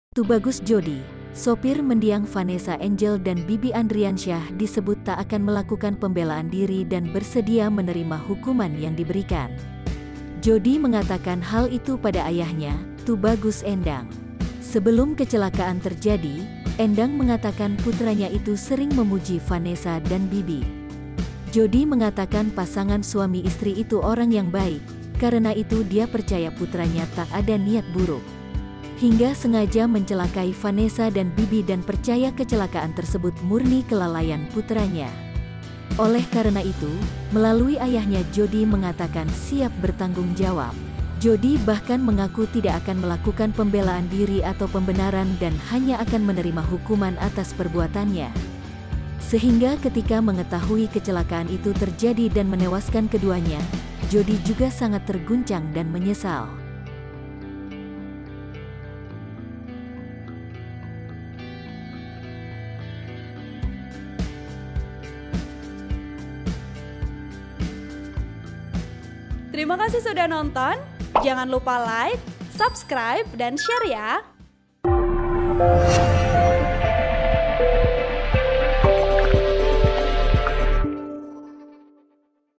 Ungkapkan Penyesalan, Tubagus Joddy Tak Akan Lakukan Pembelaan Diri
Tubagus Joddy, sopir mendiang Vanessa Angel dan Bibi Andriansyah disebut tak akan melakukan pembelaan diri dan bersedia menerima hukuman yang diberikan.

Joddy mengatakan hal itu pada ayahnya, Tubagus Endang.

Sebelum kecelakaan terjadi, Endang mengatakan putranya itu sering memuji Vanessa dan Bibi.

Joddy mengatakan pasangan suami istri itu orang yang baik, karena itu dia percaya putranya tak ada niat buruk hingga sengaja mencelakai Vanessa dan Bibi dan percaya kecelakaan tersebut murni kelalaian putranya.   (*)